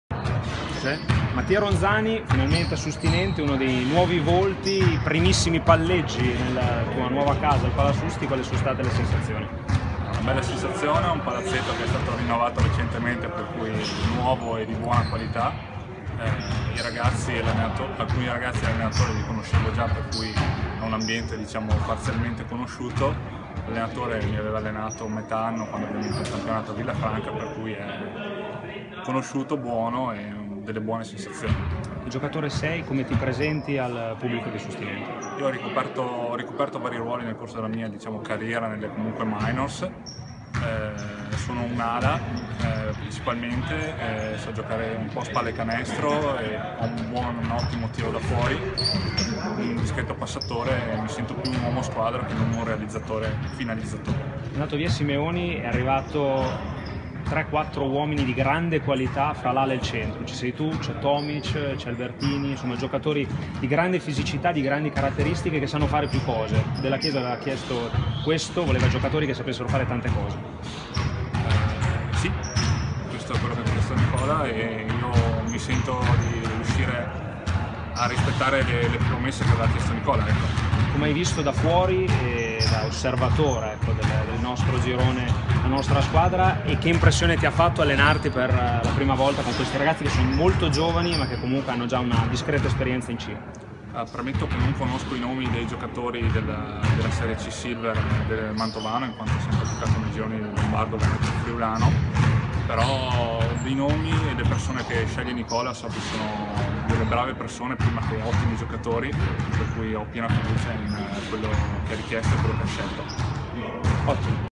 Mattia Ronzani si presenta: "Qui progetto ambizioso. Felice di poter lavorare con Della Chiesa"
Nel giorno del raduno del Sesa Sustinente è il neo arrivato Mattia Ronzani a presentarsi alla stampa. Le sue parole, cariche di emozione per questa nuova avventura, nel prossimo servizio.